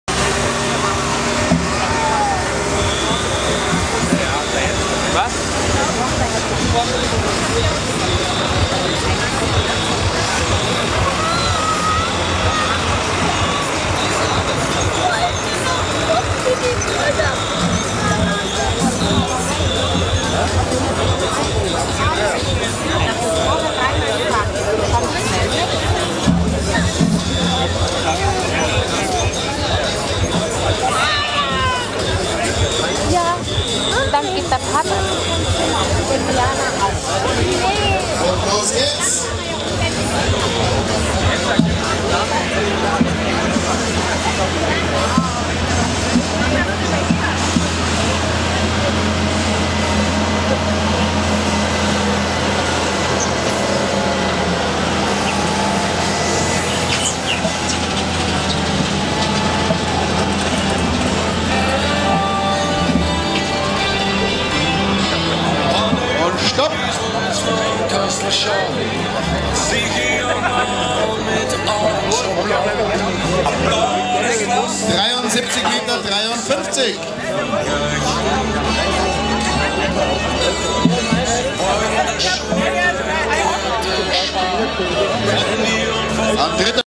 Fendt 724 / Traktorpulling Obermeitingen 2019
Hier ist ein Video vom Fendt 724 Beim Traktorpulling in Obermeitingen 2019, Ich Hoffe es gefällt euch.

Falls noch nicht passiert, Lasst doch gleich ein Abo da ;-)